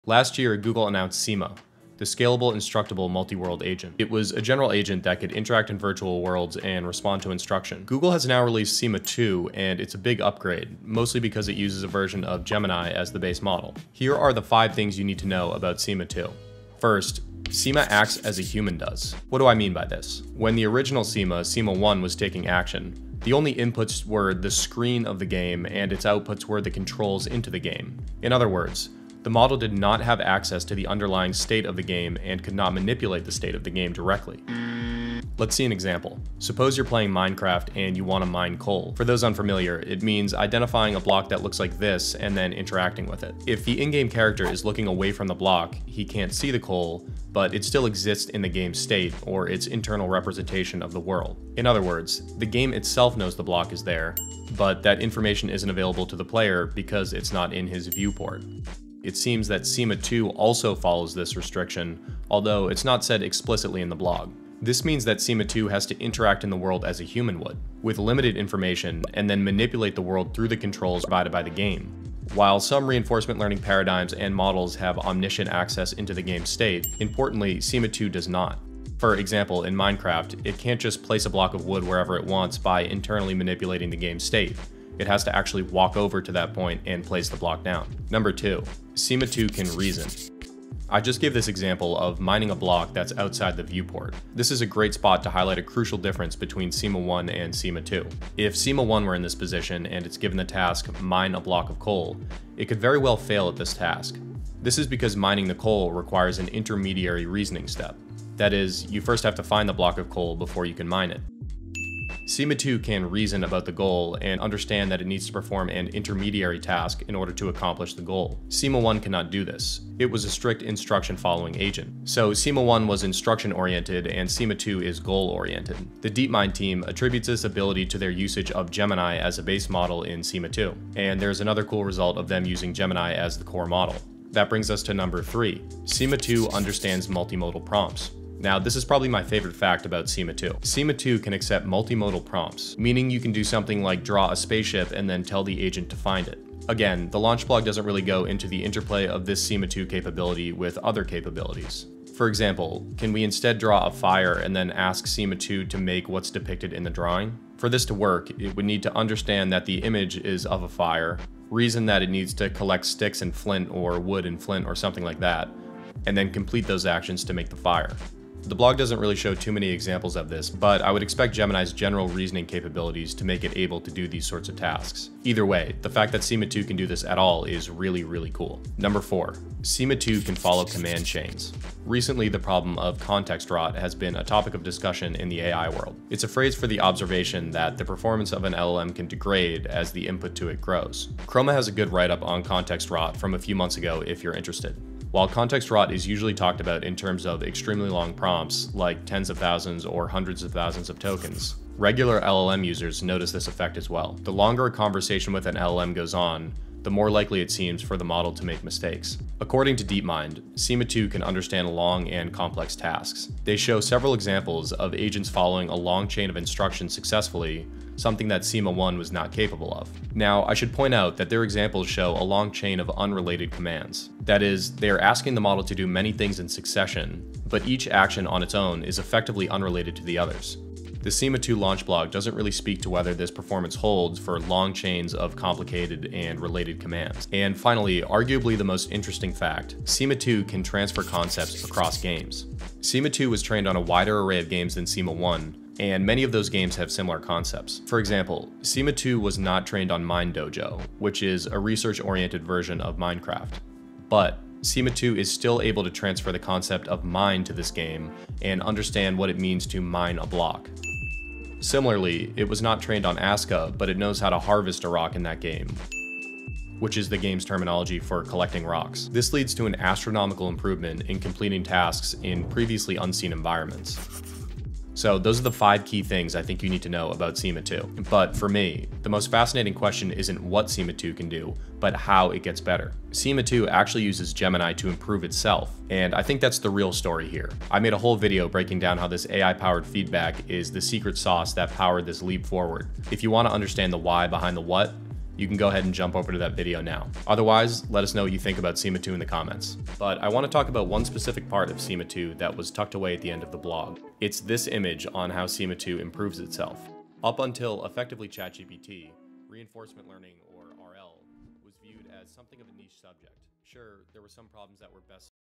Why this AI plays video games like a human (SIMA-2 Breakdown)
🎮 SIMA 2 is HERE! (5 Things You Need to Know)

Google recently dropped SIMA 2, and it’s a massive upgrade to their generalist AI agent. By using Gemini as its base model, SIMA has evolved from a simple instruction follower into a goal-oriented powerhouse capable of reasoning, planning, and adapting.

In this video, I break down the 5 crucial capabilities that set SIMA 2 apart. We look at how it interacts with games strictly like a human (no "god mode" access to game code), how it handles multimodal prompts—like finding an object based on a drawing—and its incredible ability to transfer concepts like "mining" to games it has never seen before.

Which of these 5 features do you think is the biggest leap for AI agents?

👩‍💻 We're Hiring! See open roles
Timestamps
0:00 - Google announces SIMA 2
0:16 - 1. It acts like a human
1:28 - 2. Reasoning: Goal-oriented vs Instruction-oriented
2:15 - 3. Understanding Multimodal Prompts
2:57 - 4. Following long Command Chains
3:59 - 5. Transferring concepts across games
4:36 - The real story: How SIMA 2 improves itself